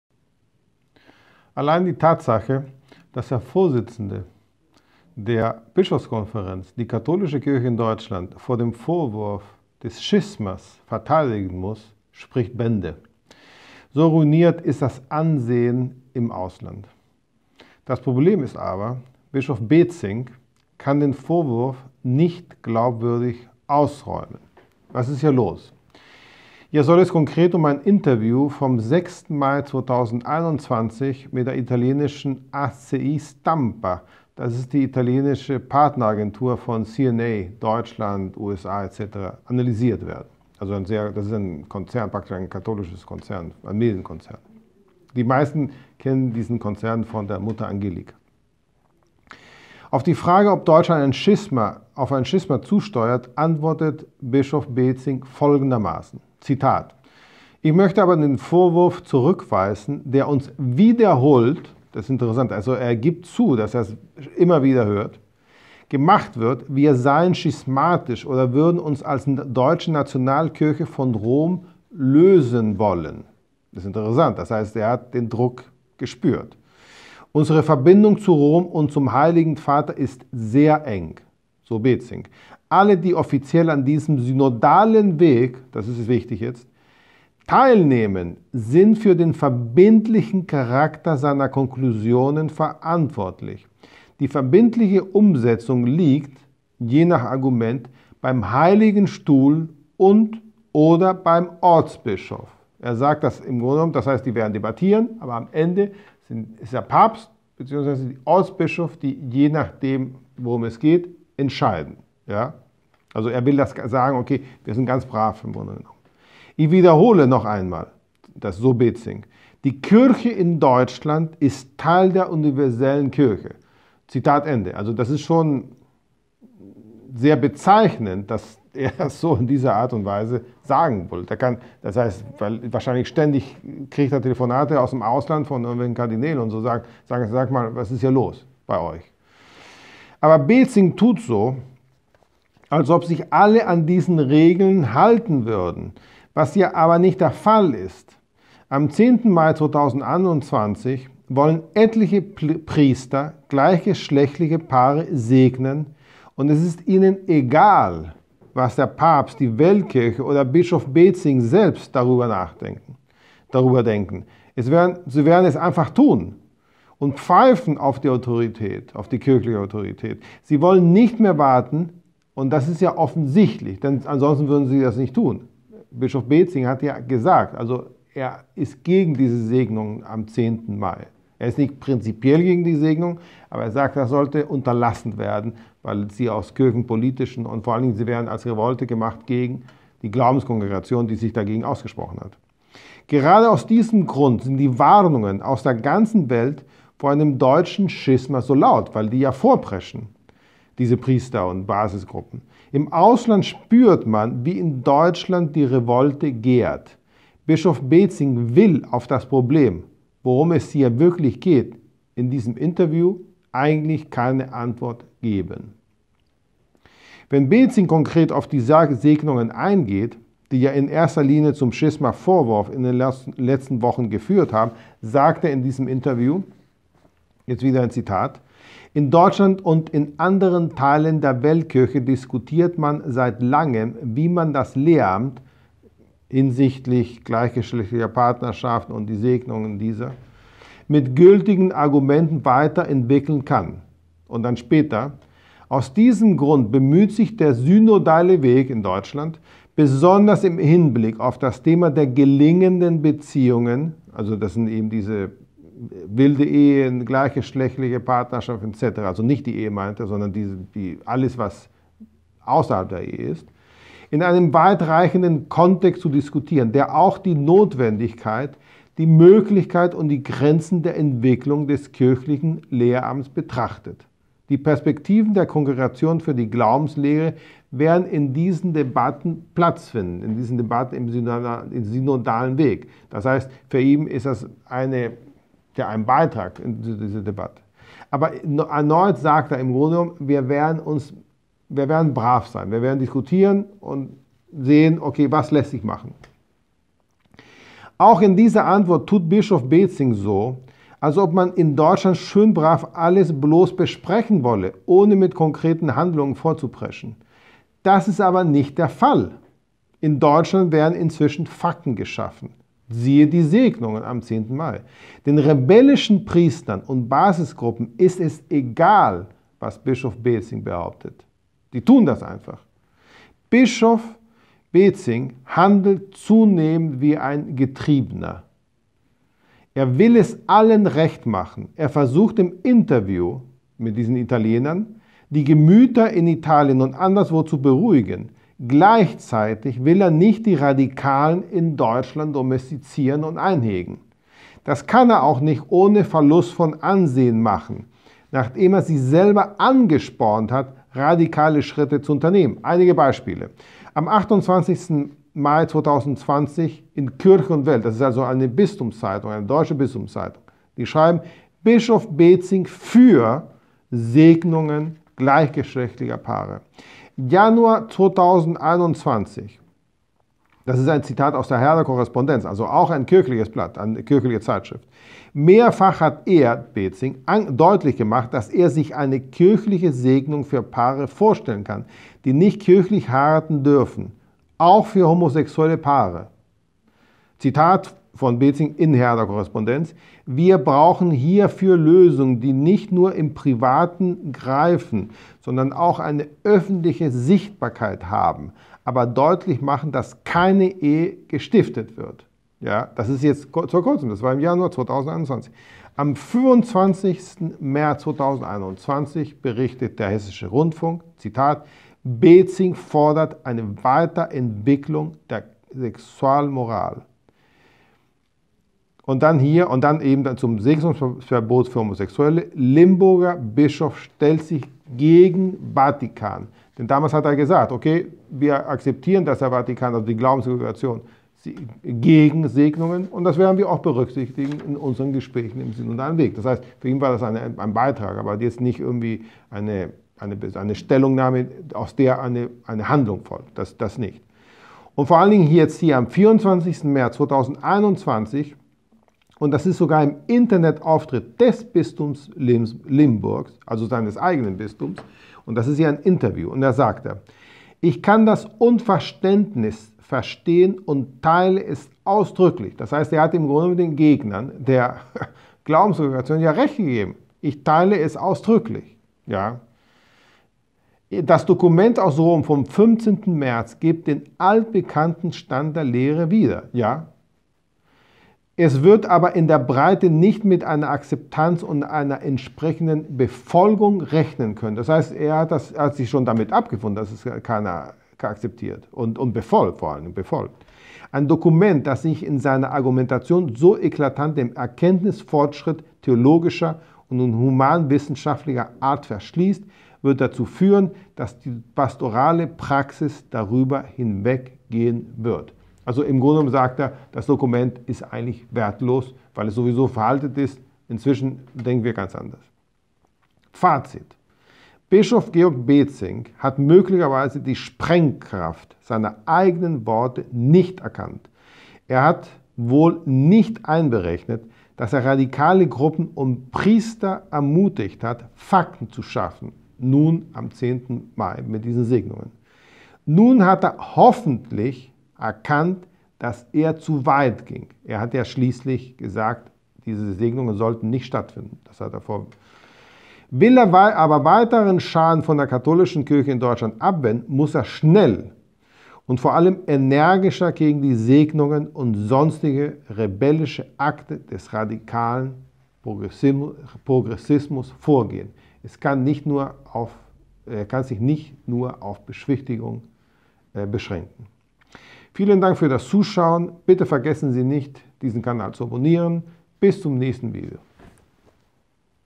Schisma-Vorwurf wegen Segnungen: Bätzing hat sich verrannt
Allein die Tatsache, dass der Vorsitzende der Bischofskonferenz die katholische Kirche in Deutschland vor dem Vorwurf des Schismas verteidigen muss, spricht Bände. So ruiniert ist das Ansehen im Ausland. Das Problem ist aber: Bischof Bätzing kann den Vorwurf nicht glaubwürdig ausräumen.
Bischof Georg Bätzing handelt zunehmend wie ein Getriebener. Er will es allen recht machen. Er versucht im Interview die Gemüter in Italien und anderswo zu beruhigen. Gleichzeitig will er nicht die Radikalen in Deutschland domestizieren und einhegen.